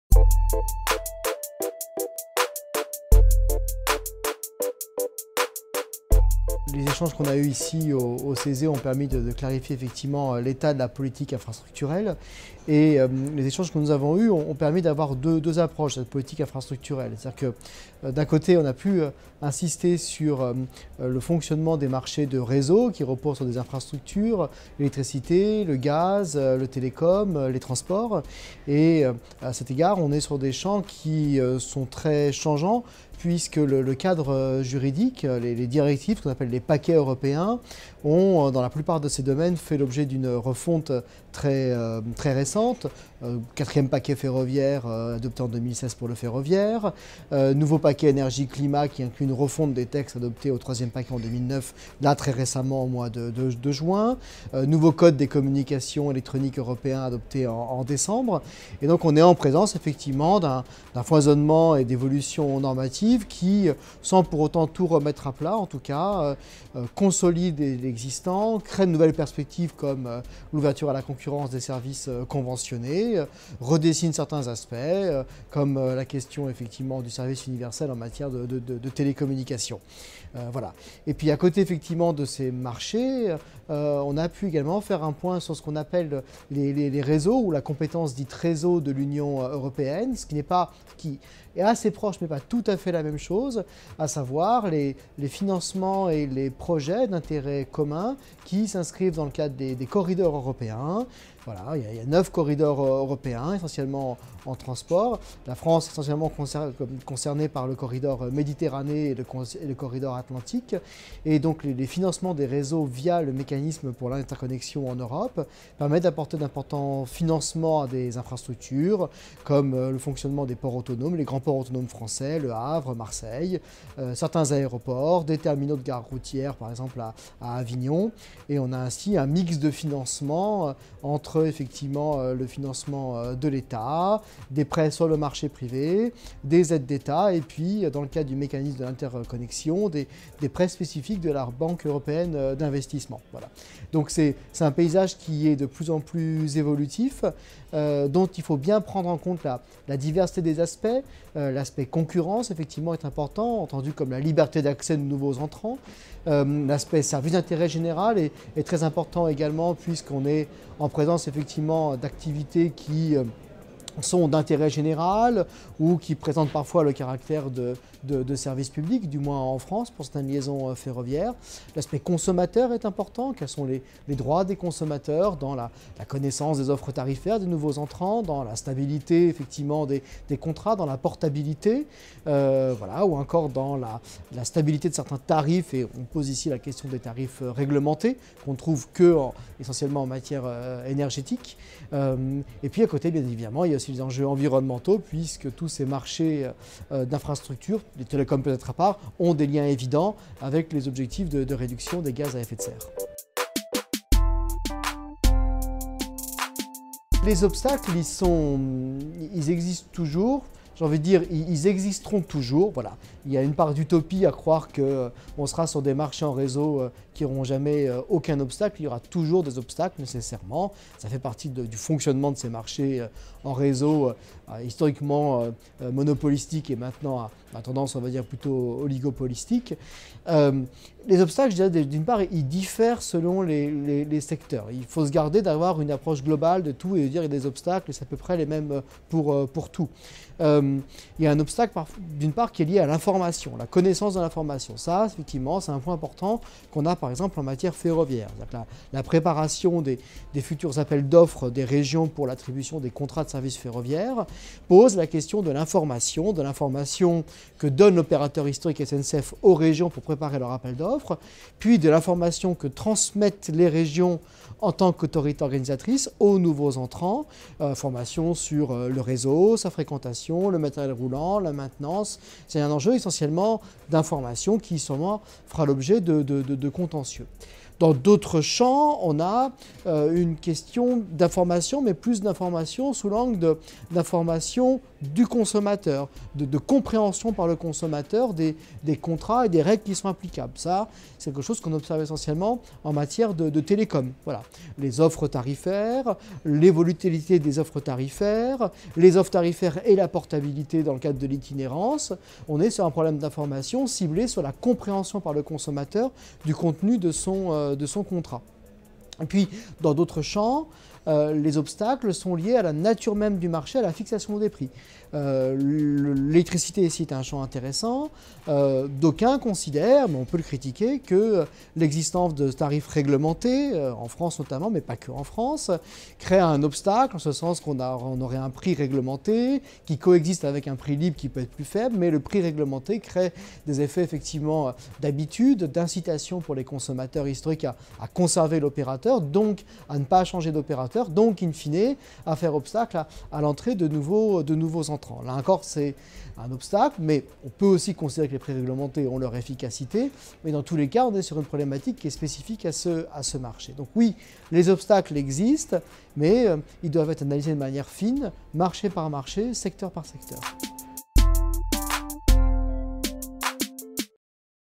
Stéphane de LA ROSA (Université Paris-Est Créteil) - infrastructures de réseaux - cese
Questions à M. Stéphane de LA ROSA, professeur des Universités, Directeur de l'équipe de recherche Marchés, institutions, libertés (MIL) à l'Université Paris-Est Créteil, auditionné par la Section des activités économiques du CESE dans le cadre de la saisine "L'impact des infrastructures de réseaux dans l'économie".